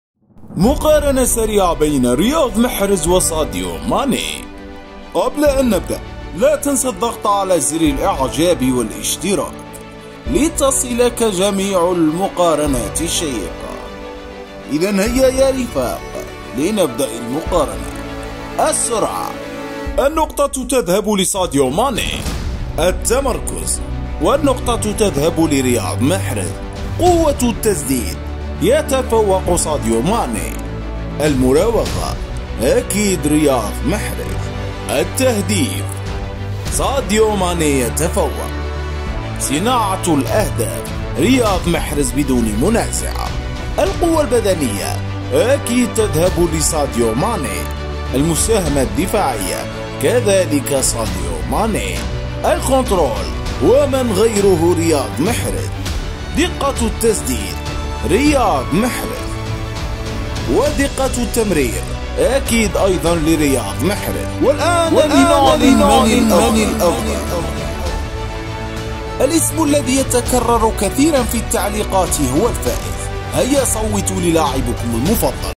مسابقة بين رياض محرز و ساديو ماني 🤔 لن تصدق من سيفوز فالآخير 😱 هذا هو الأفضل بالدليل 🇩🇿🇸🇳
مقارنة قوية بين ساديو ماني ورياض محرز لن تصدق من سيفوز فالآخير 🔥🔥🇸🇳🇩🇿  تحركات ومزايا قائد المنتخب الجزائري رياض محرز ونجم مانشستر سيتي ❤️ ضد قائد ساديو ماني نجم منتخب السنغال ونجم ليفربول سابقا وبايرن ميونخ حاليا  🔥